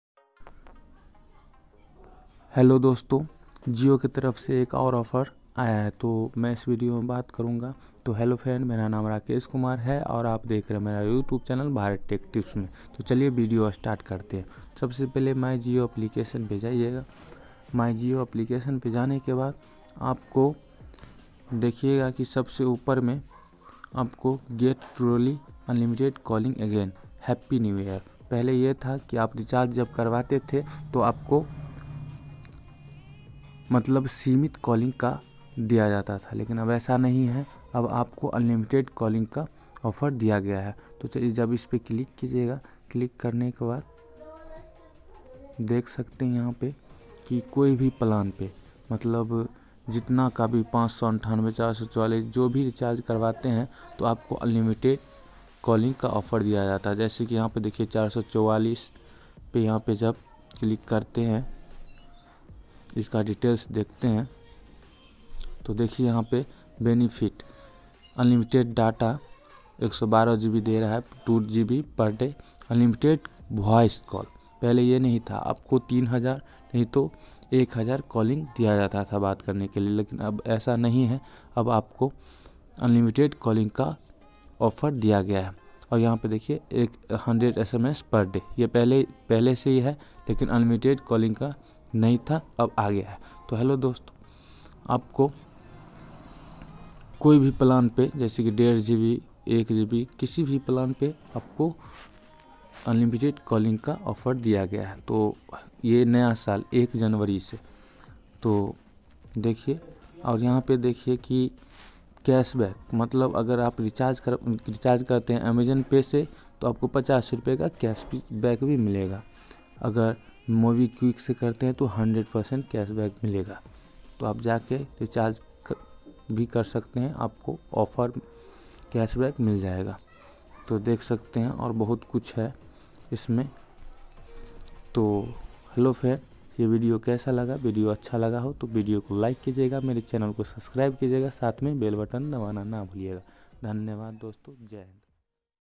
Jio Offer||Jio Offer 2021||New Year Jio Offer||Jio Offer Today||Bharat Tech Tips
Hello Friend
                Mai Is Video Me Bataunga Ki Jio Ka Labh Kaise  Uthaye.Mere Is Chanal Par Tech Treated Video Upload Ki Jati Hai.
Agar Mera Video Pasand Aaye To Mere Ko Like Jaroor Kijiyega Dhanybad.

Jio Offer||Jio Offer 2021||New Year Jio Offer||Jio Offer Ka Labh Kaise Uthaye||Bharat Tech Tips

    Rakesh Kumar